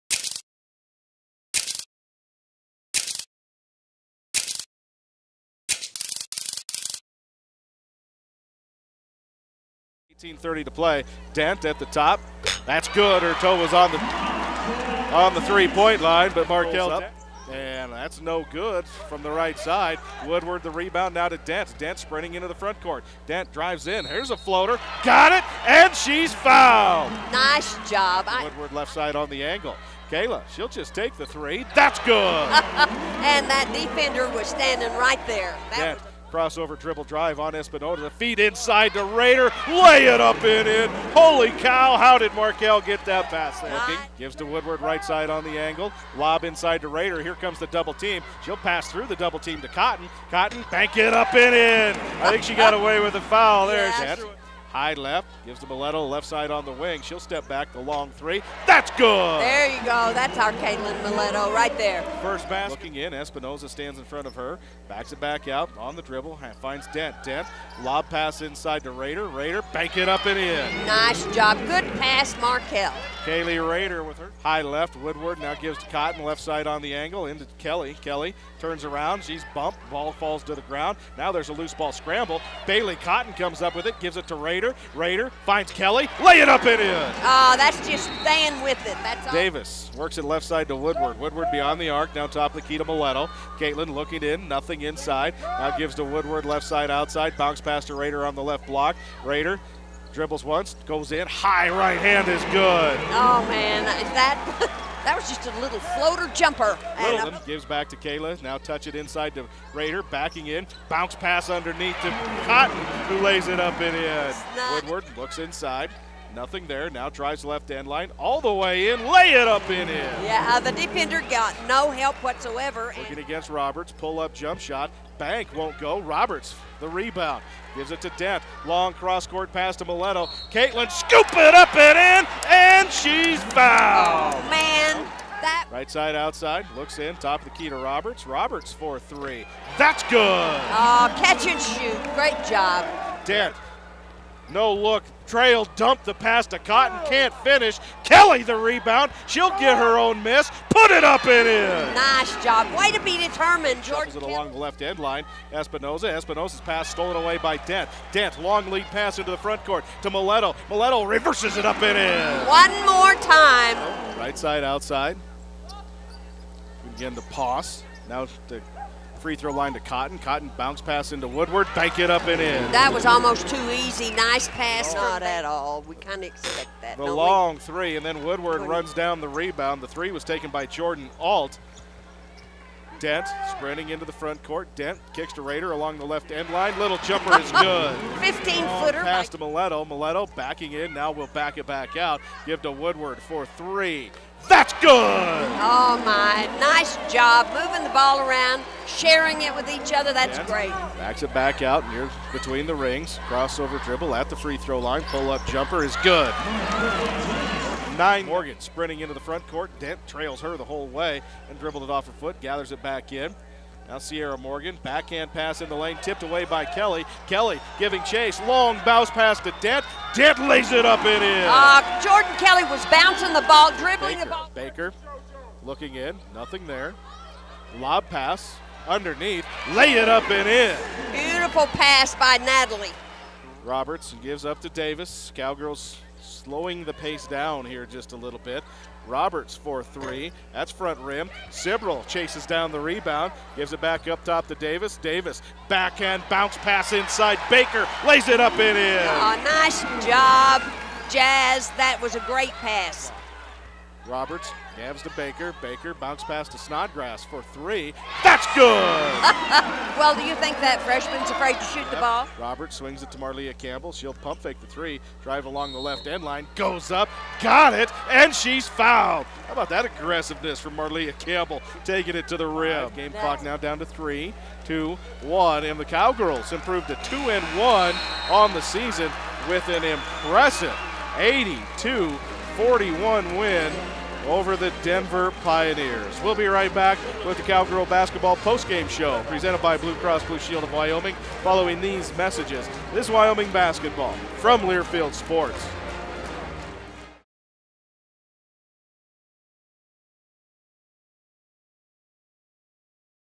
11.21.14 Women's Basketball vs. Denver Highlights
Highlights from the Cowgirls 82-41 win over Denver on 11.21.14 at the Arena-Auditorium.